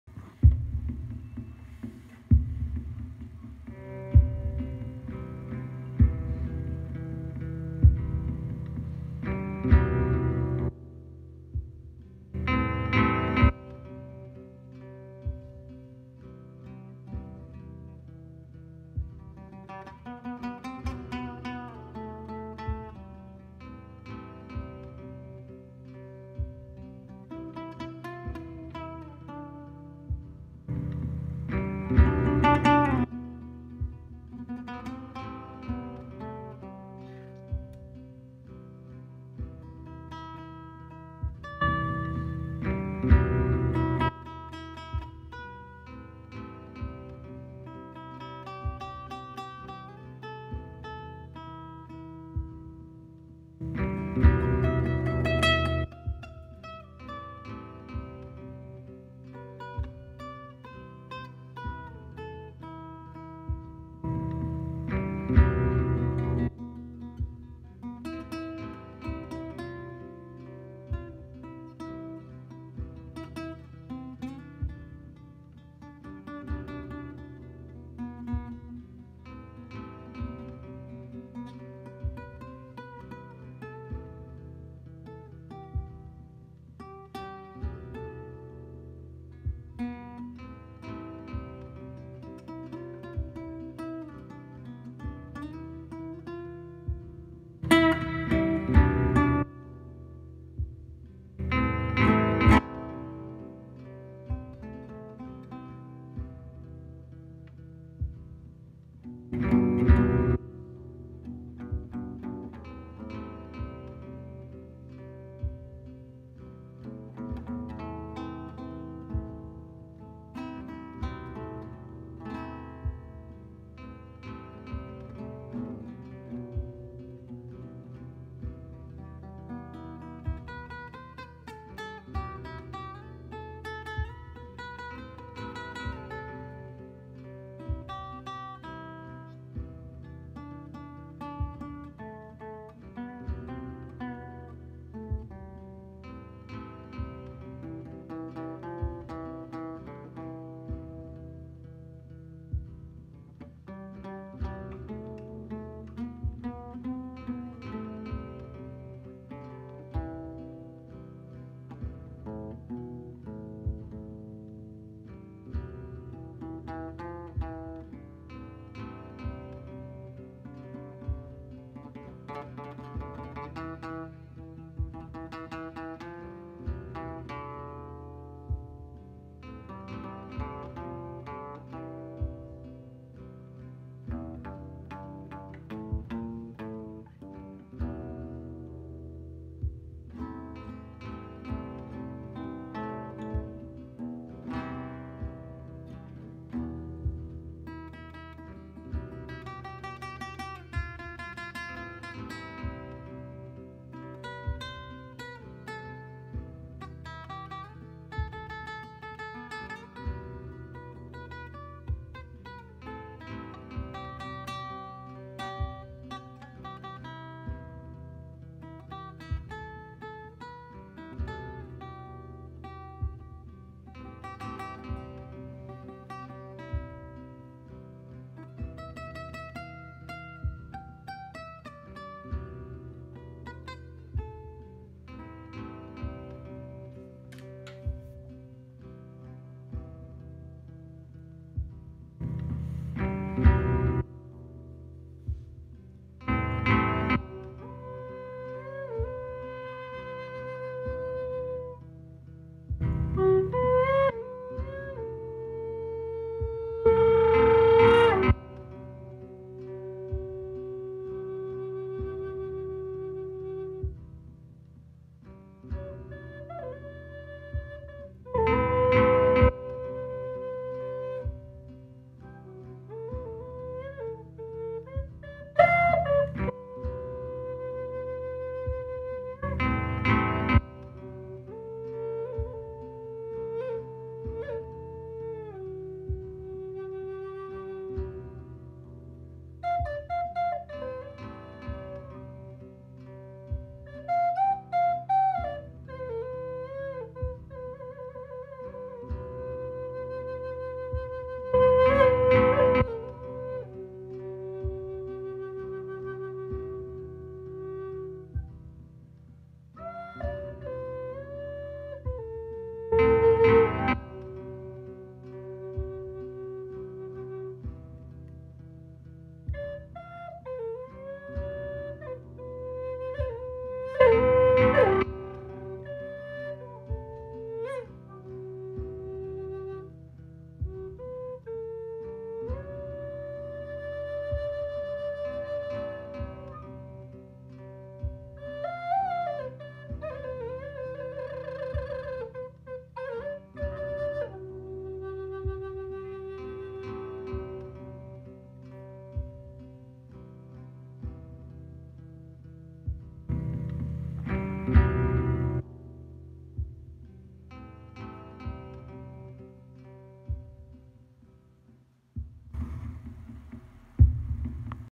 Nice chill loop 432hz live looping
Solo improv over an improv loop that just came together sweeeetly into a hypnotic spiral of sounds of one’s soul 🧙‍♂️💖🙏☮️🎶🧬